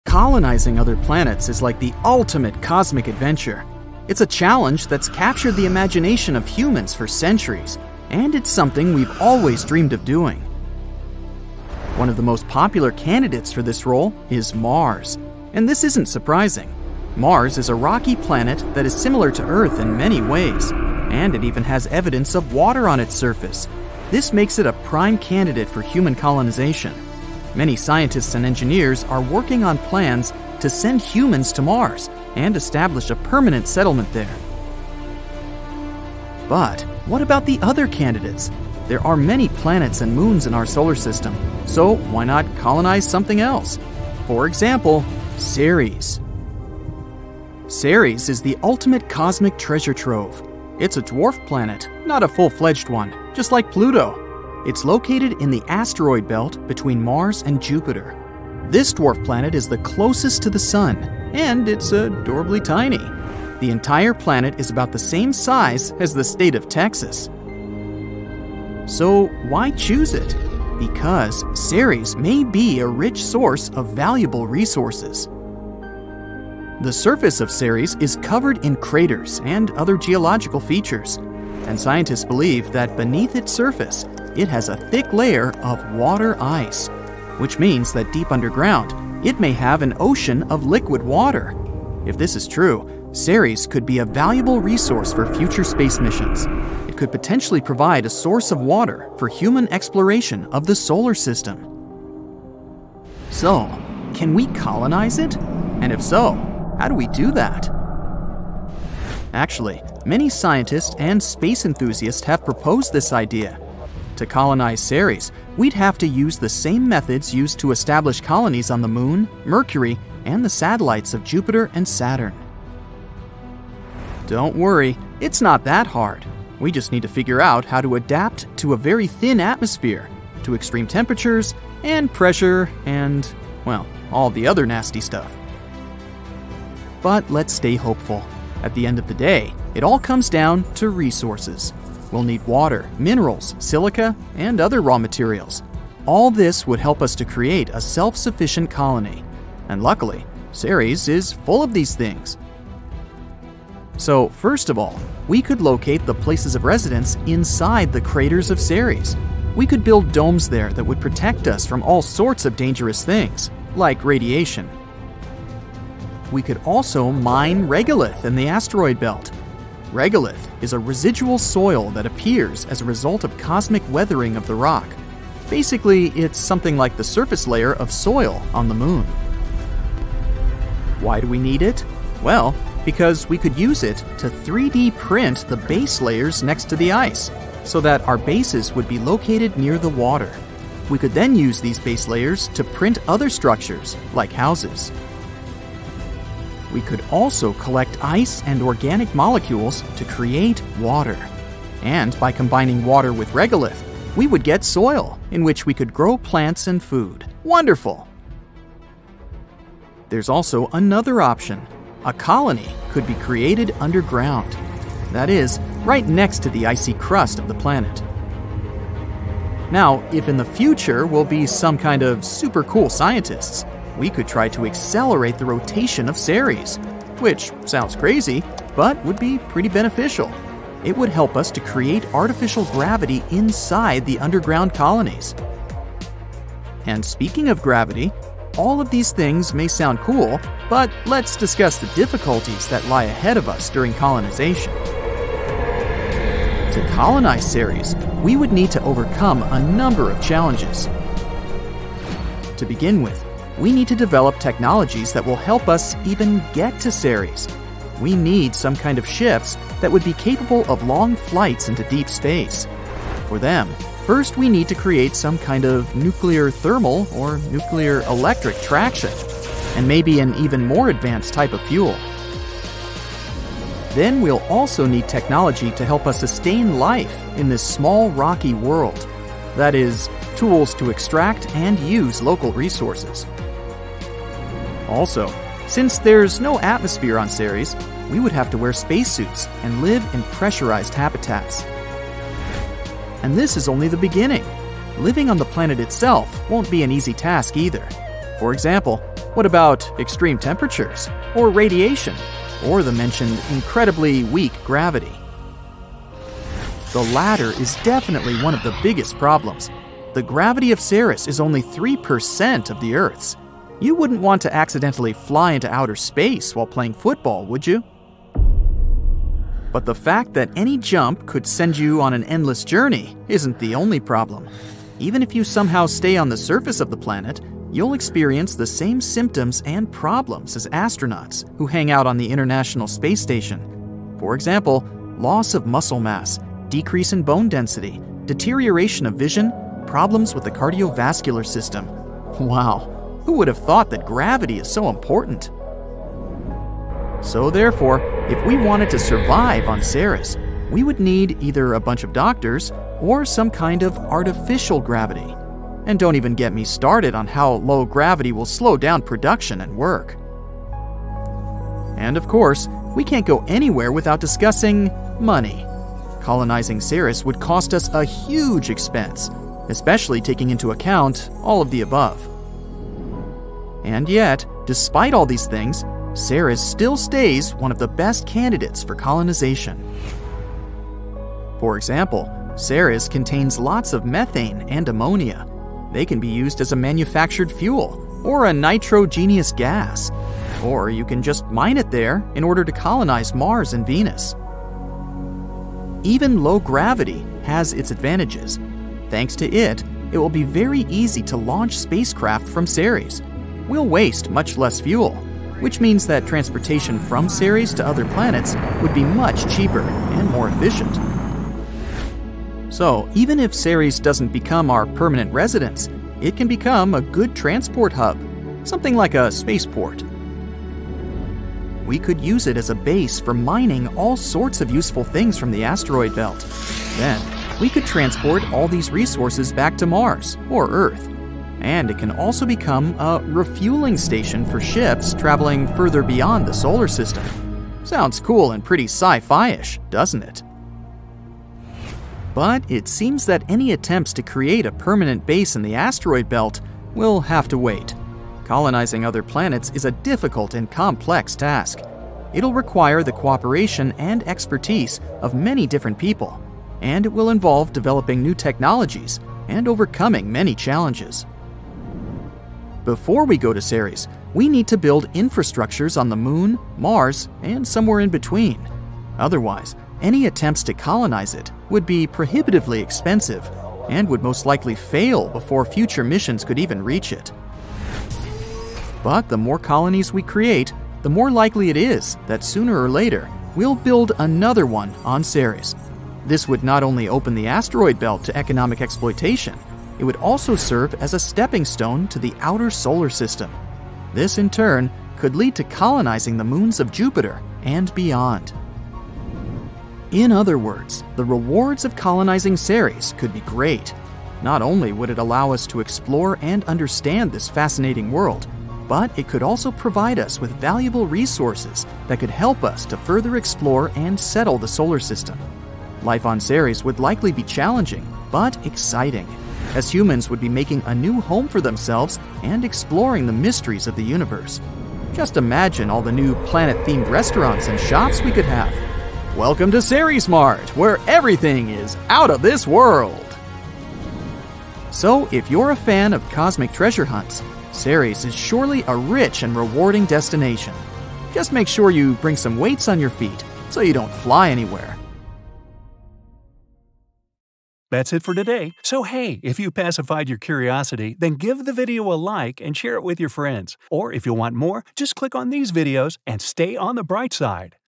Forget Mars, This Tiny Planet Is Better for Colonization
Will humans ever relocate to some other planet in the Solar System? One of the most popular candidates for this role is Mars, but what about other options? Ceres is the ultimate cosmic treasure trove. It's a dwarf planet, not a full-fledged one — just like Pluto. It’s located in the asteroid belt between Mars and Jupiter. This dwarf planet is the closest to the Sun, and it’s adorably tiny - sounds good so far.